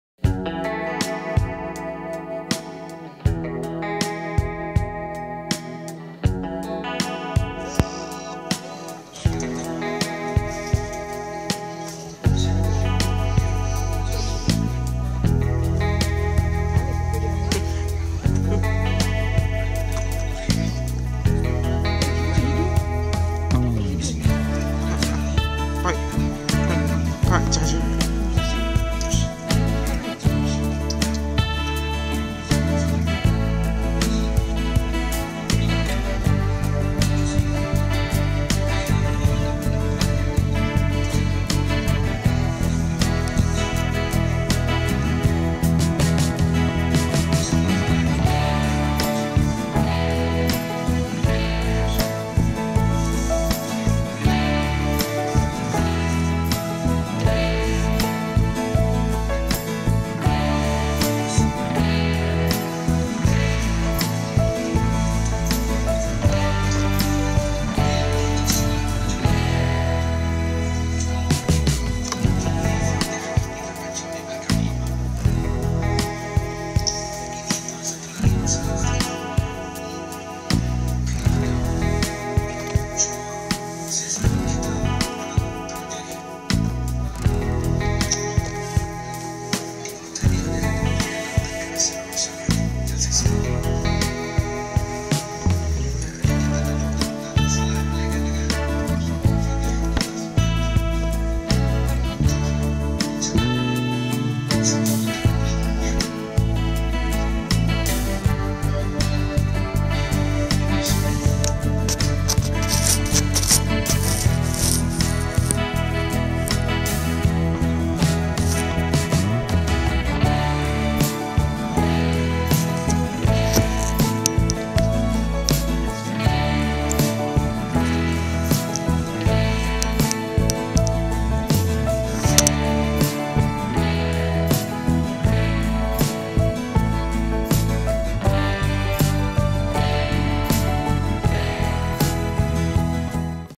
The Crazy Cat Season 2 Episode 2
This is the second episode of The Crazy Cat Season 2. Kecik bites the charging wire.